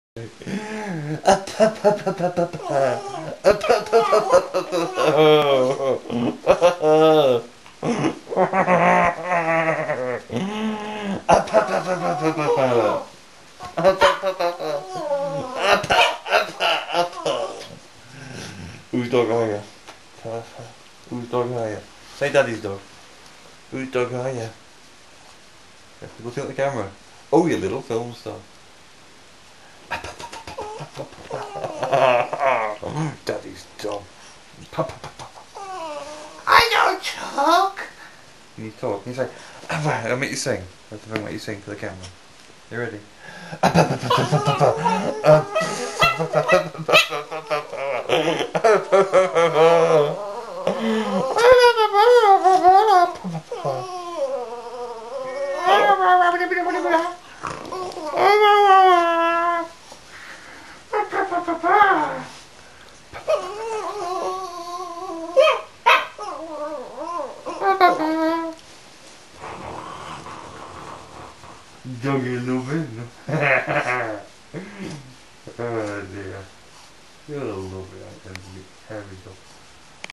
THE CUTEST DOG IN THE WORLD SINGS A SONG TO DADDY
He is cute, fluffy, and musically talented